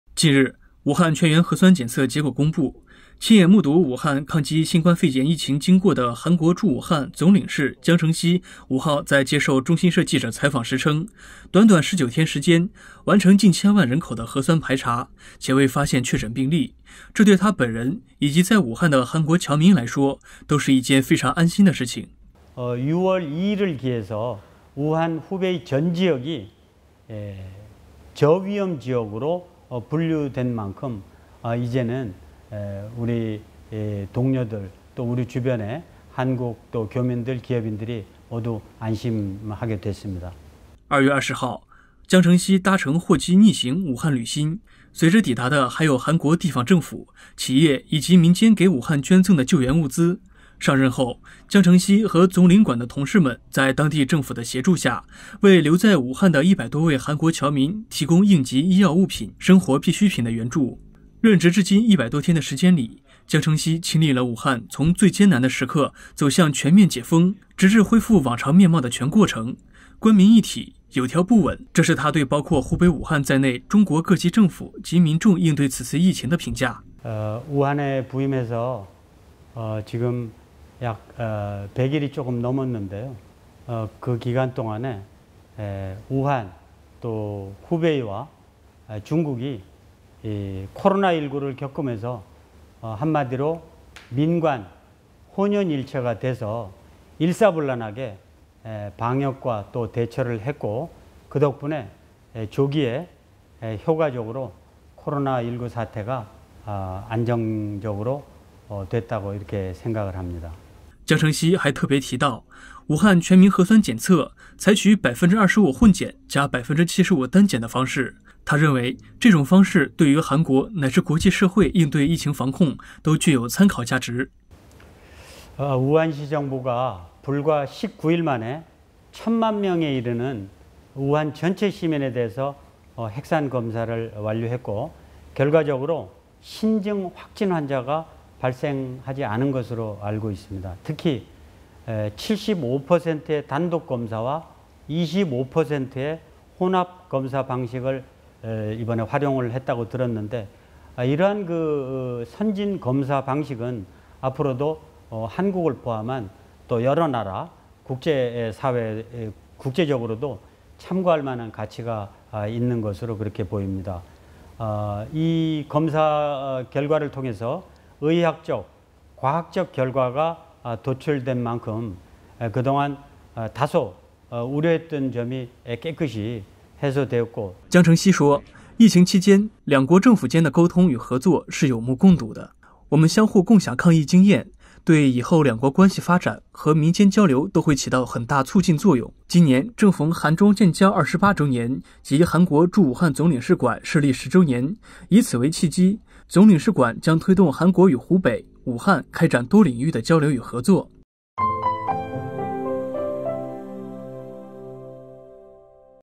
韩国驻武汉总领事姜承锡：我在武汉很安心
近日，武汉全员核酸检测结果公布，亲眼目睹武汉抗击新冠肺炎疫情经过的韩国驻武汉总领事姜承锡称，短短19天时间，完成近千万人口的核酸排查，且未发现确诊病例，这对他本人以及在武汉的韩国侨民来说，都是一件非常安心的事情。任职至今100多天的时间里，姜承锡亲历了武汉从“最艰难”的时刻走向全面“解封”，直至恢复往常面貌的全过程。“官民一体，有条不紊”，这是他对包括湖北武汉在内中国各级政府及民众应对此次疫情评价。